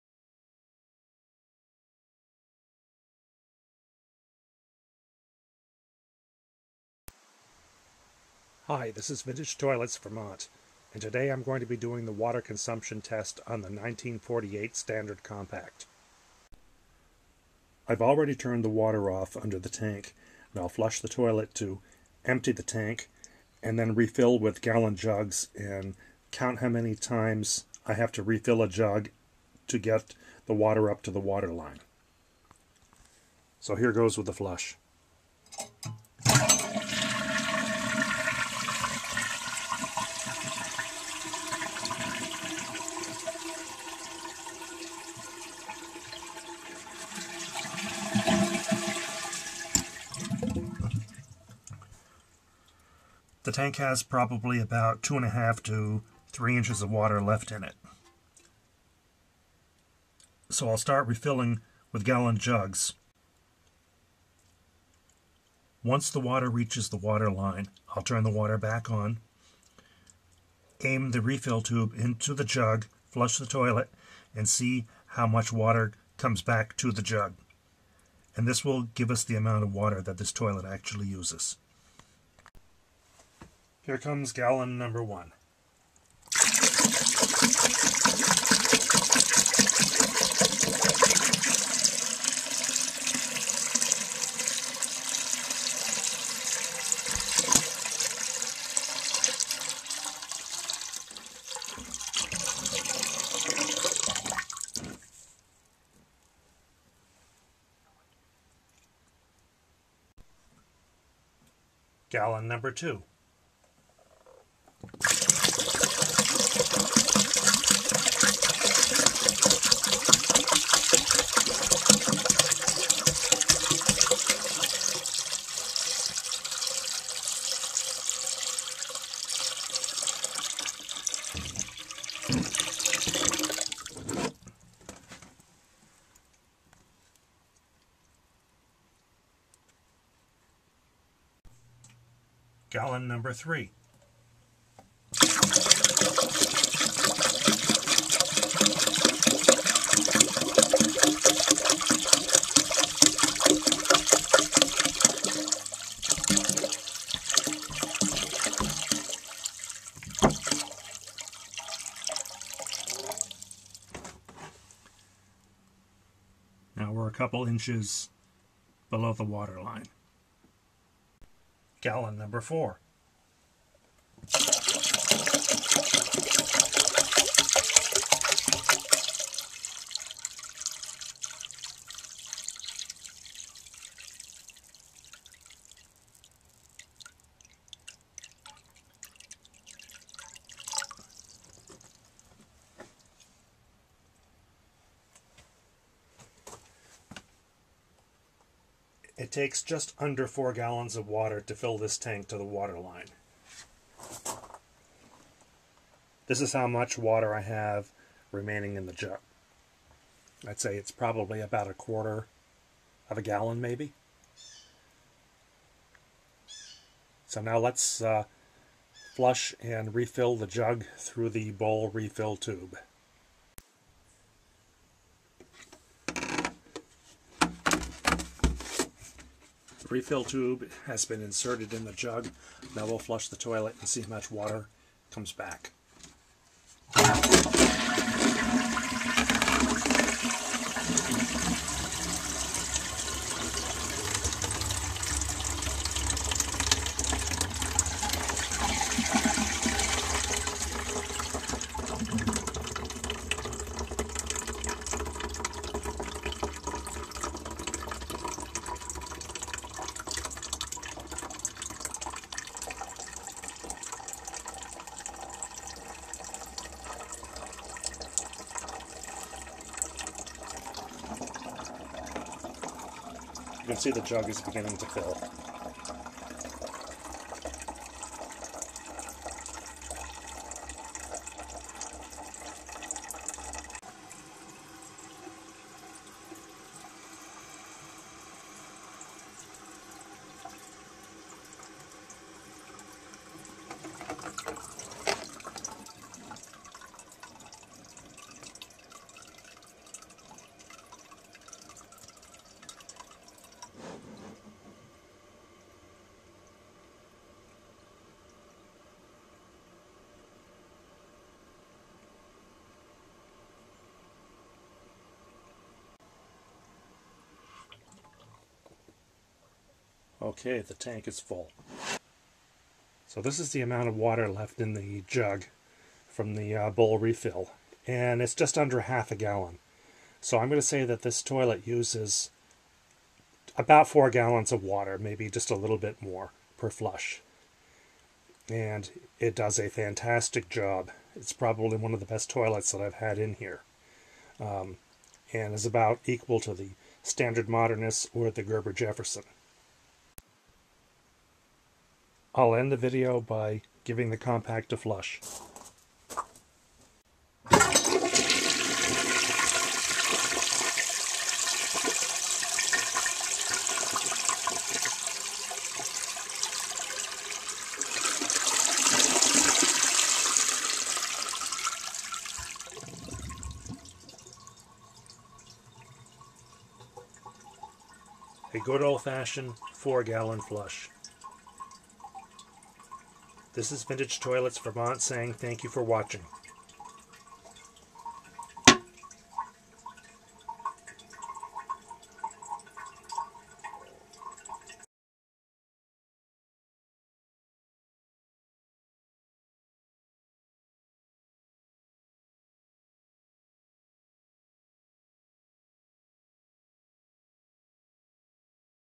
76. 1948 "Standard" Compact Water Usage Test
The 1948 "Standard" Compact is given the water usage test.  The tank is flushed with the water off then refilled with gallon jugs.  Then the water is turned back on, the bowl refill tube inserted into the jug, and the toilet flushed.  The water in the tank + water in the jug after the manual fill + water in the jug from the bowl refill tube = the approximate water usage of the toilet.